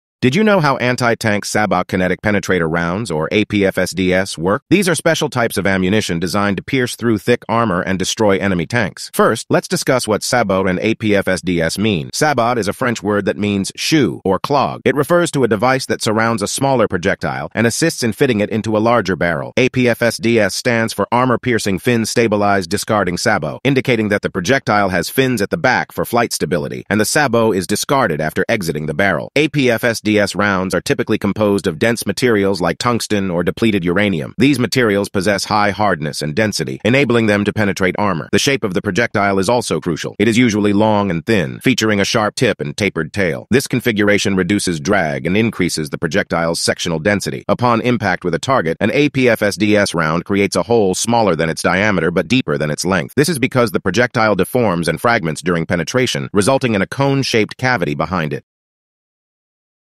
How Do APFSDS Rounds Work?
Have you ever wondered how APFSDS rounds penetrate enemy armor? In this video, we delve into the fascinating world of Armor-Piercing Fin-Stabilized Discarding Sabot (APFSDS) rounds.